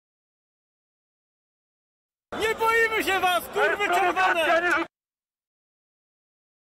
Nie boimy się was .... :)
Przepraszam ale padł bardzo ciekawy komentarz na 'marszu niepodległości 2013', że od razu mi się skojarzyło zrobienie parodii z tego zaś komentarza :)

odsyłam was do oryginalnego zresztą mojego filmu :)